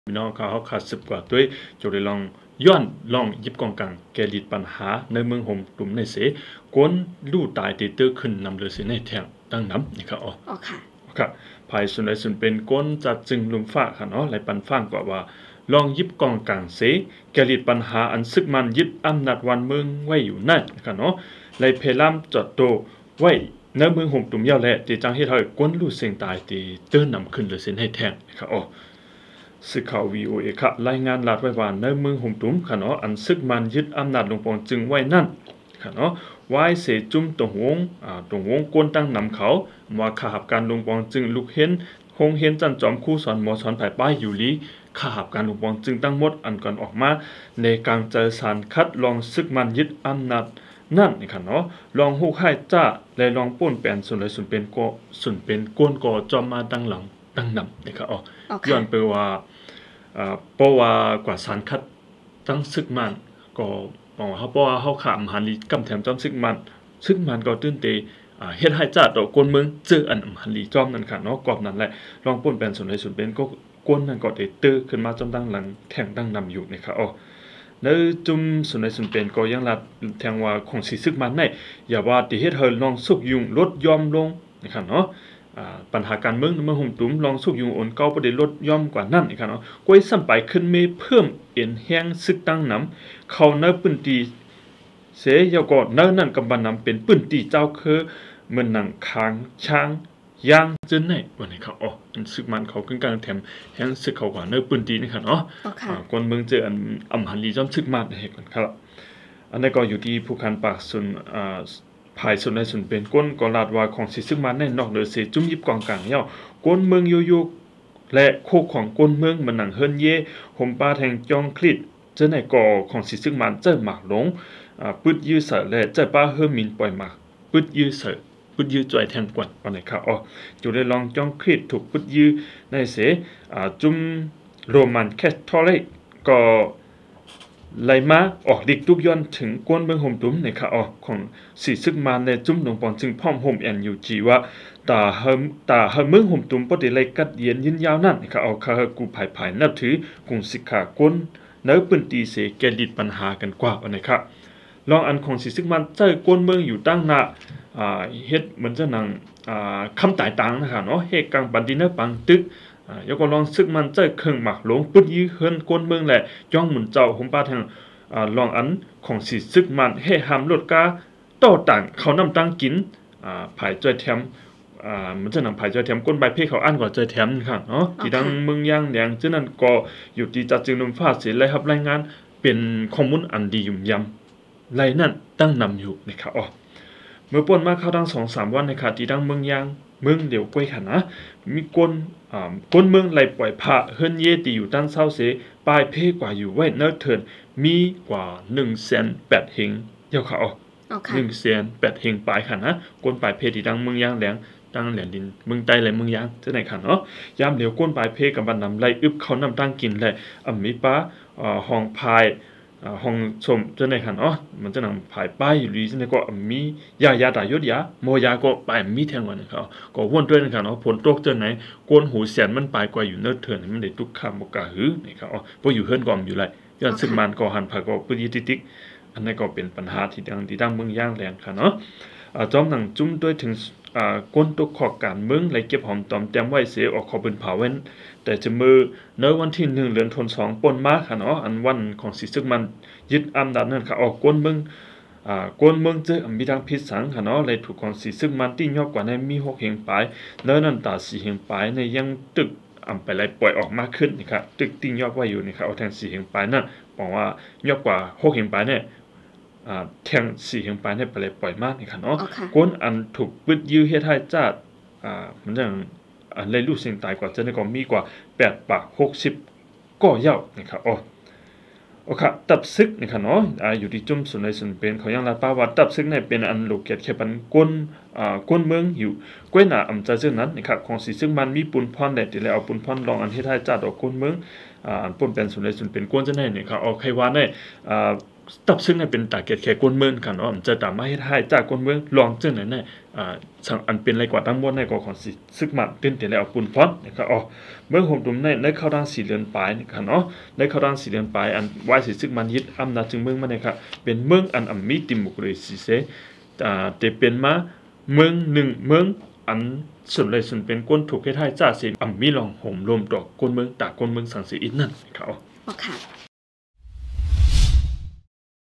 TaiFreedom Video 13.06.2021 ယွၼ်ႉလွင်ႈယိပ်းၵွင်ႈၵၢင်ၵႄႈလိတ်ႈပၼ်ႁႃယူႇၼႂ်းမိူင်းႁူမ်ႈတုမ်ၼႆႉ ၵူၼ်းလူႉ
1)ယွၼ်ႉလွင်ႈယိပ်းၵွင်ႈၵၢင်ၵႄႈလိတ်ႈပၼ်ႁႃယူႇၼႂ်းမိူင်းႁူမ်ႈတုမ်ၼႆႉ ၵူၼ်းလူႉတၢႆတေတိူဝ်းၶိုၼ်ႈထႅင်ႈ